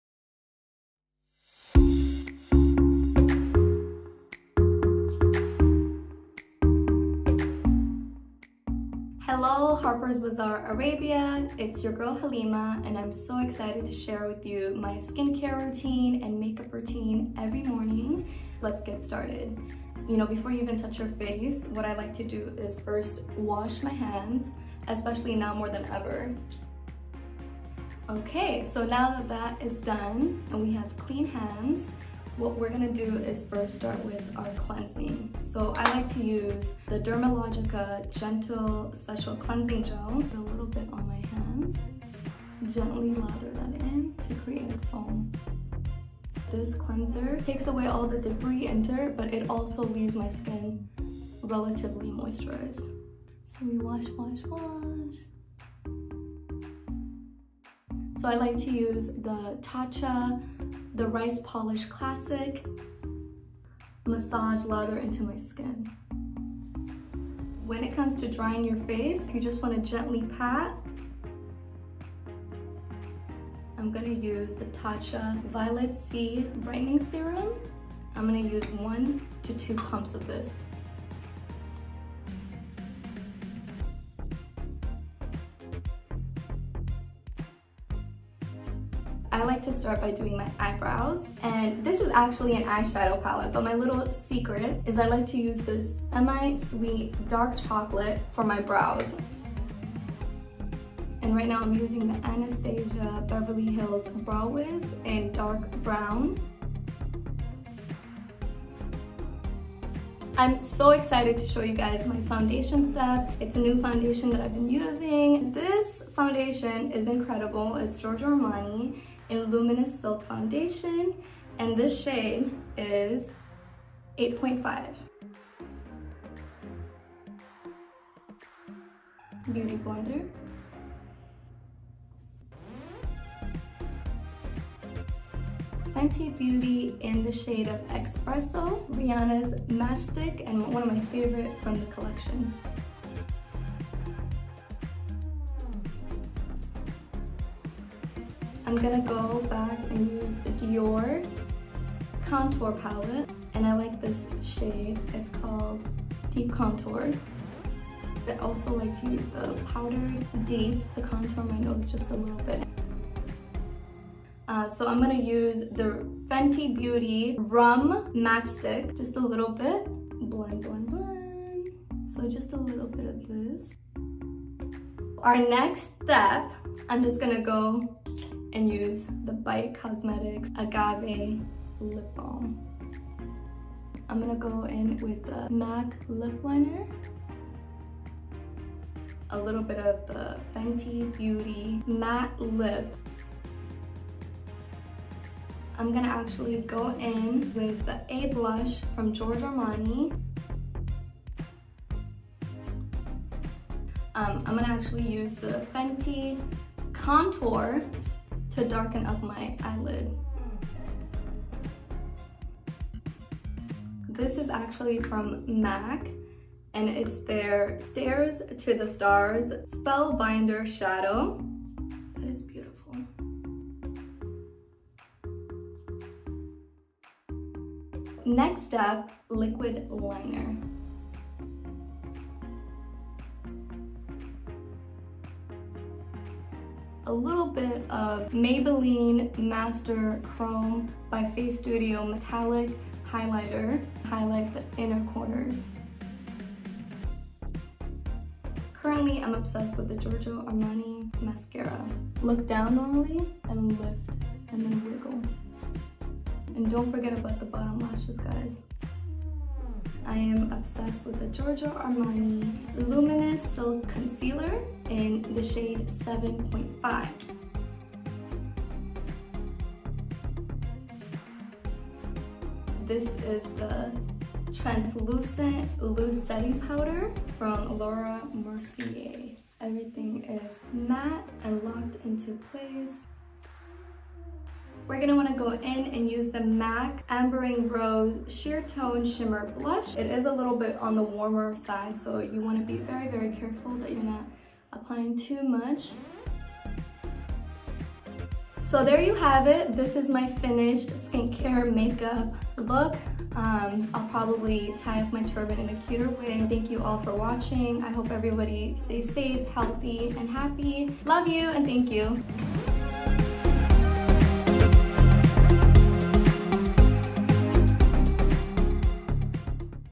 Why Halima Aden Swears By Tatcha Rice Polish Classic
Hijabi supermodel and Feb 2020 Harper’s Bazaar Arabia cover star Halima Aden shares her morning routine on IWokeUpLikeThis. From keeping her skin looking fresh with the help of Tatcha, Dermalogica and Augustinus Bader products to creating a killer contour with Fenty Beauty’s contour stick she shows us all the tricks. Watch it all on Harper’s Bazaar Arabia’s IWokeUpLikeThis.